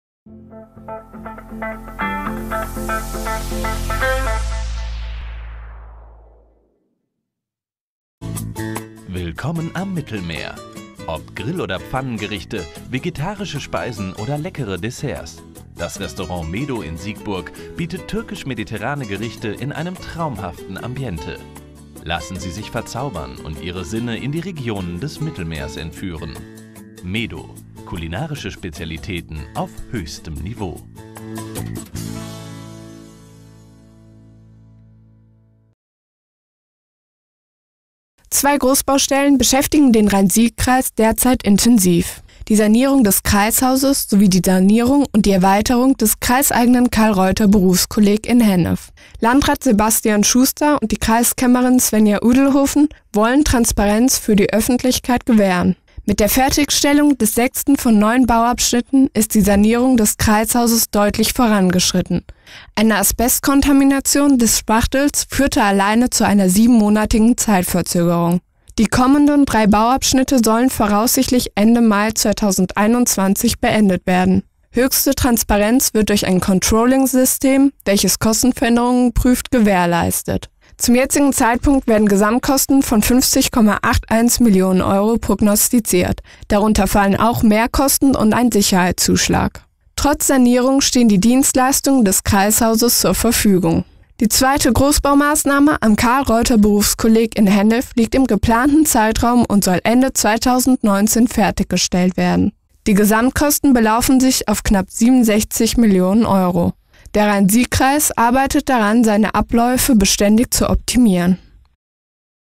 Sanierungsmaßnahmen des Rhein-Sieg-Kreises schreiten voran
Zwei Großbaustellen beschäftigen den Rhein-Sieg-Kreis derzeit intensiv – die Sanierung des Kreishauses sowie die Sanierung und die Erweiterung des kreiseigenen Carl-Reuther-Berufskolleg in Hennef.

Landrat Sebastian Schuster und die Kreiskämmerin Svenja Udelhoven wollen Transparenz für die Öffentlichkeit gewähren.

Mit der Fertigstellung des sechsten von neun Bauabschnitten ist die Sanierung des Kreishauses deutlich vorangeschritten. Eine Asbestkontamination des Spachtels führte alleine zu einer sieben-monatigen Zeitverzögerung. Die kommenden drei Bauabschnitte sollen voraussichtlich Ende Mai 2021 beendet werden. Höchste Transparenz wird durch ein Controllingsystem, welches Kostenveränderungen prüft, gewährleistet. Zum jetzigen Zeitpunkt werden Gesamtkosten von 50,81 Millionen Euro prognostiziert, darunter fallen auch Mehrkosten und ein Sicherheitszuschlag. Trotz Sanierung stehen die Dienstleistungen des Kreishauses zur Verfügung.

Die zweite Großbaumaßnahme am Carl-Reuther-Berufskolleg in Hennef liegt im geplanten Zeitraum und soll Ende 2019 fertig gestellt werden. Die Gesamtkosten belaufen sich auf knapp 67 Millionen Euro.

Der Rhein-Sieg-Kreis arbeitet daran, seine Abläufe beständig zu optimieren.